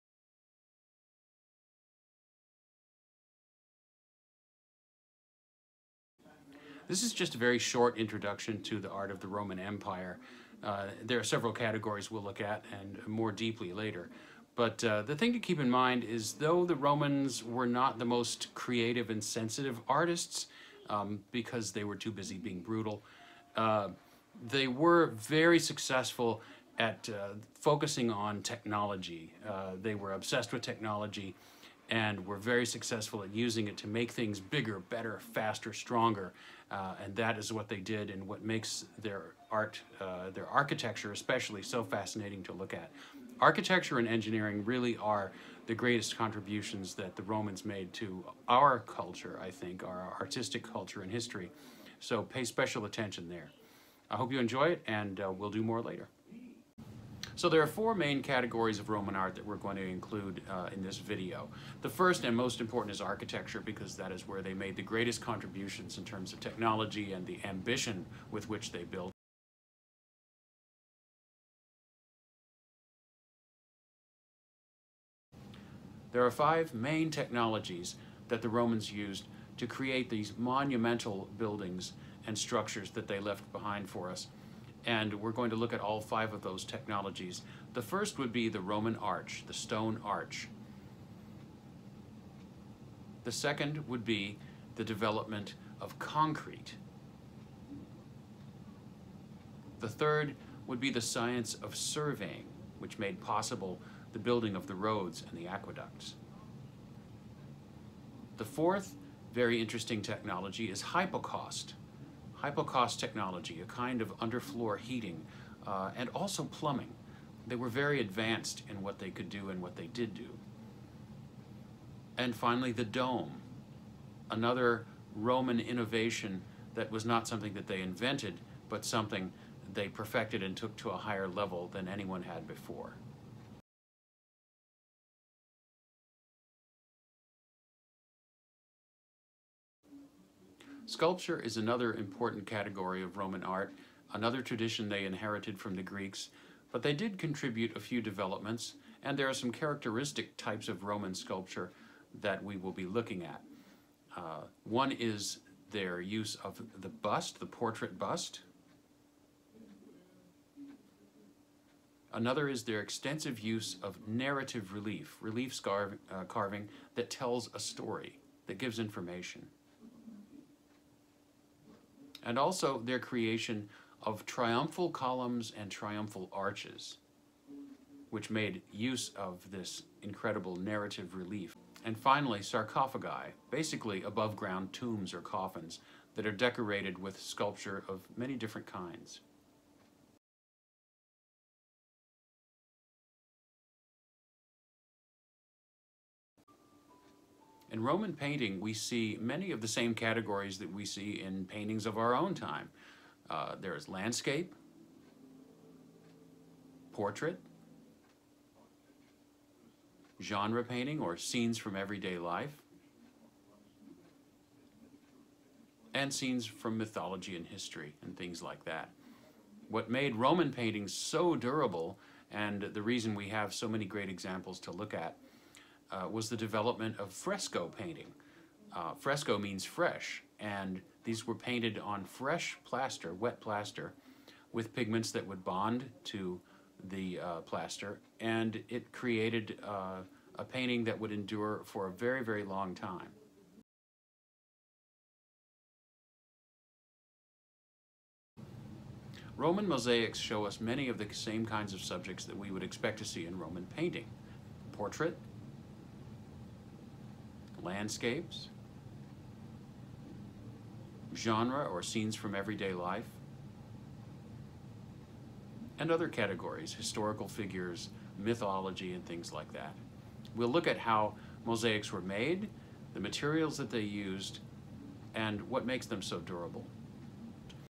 Roman Art Overview
A very (very) brief introduction to the art of the Roman Empire, comprising four sections touching on four important categories of Roman Art.